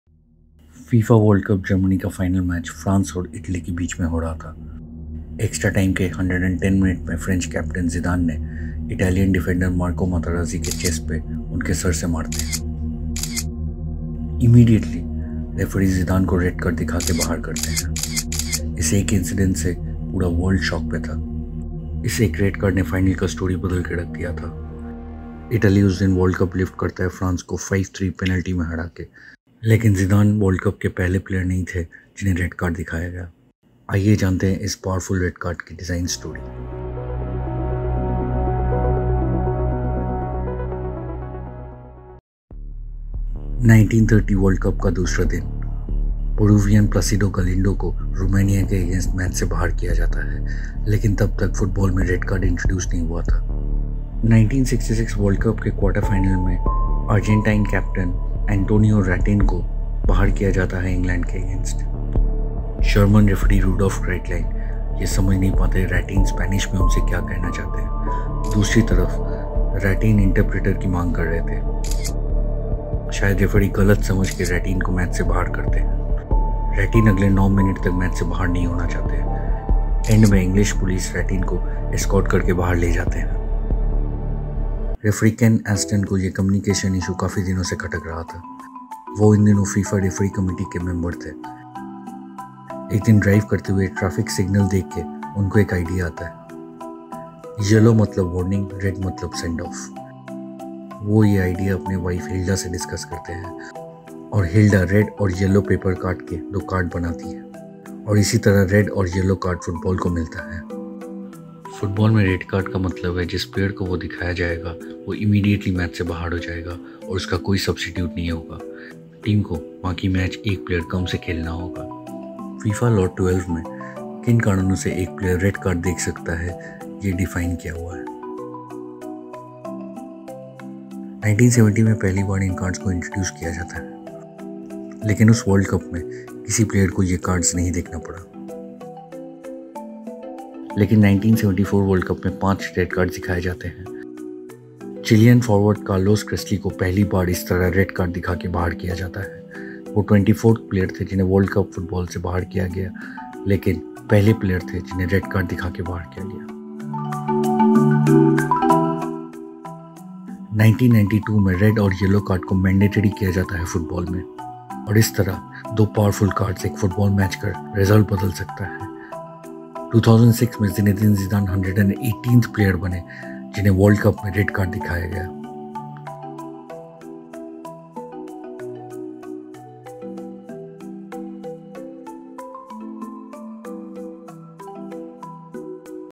How RED CARD changed soccer
The referee’s red and yellow cards are vital pieces of the official’s armory. What would a referee be without them? Well, there was a time, not so long ago, that referees had to issue discipline without them.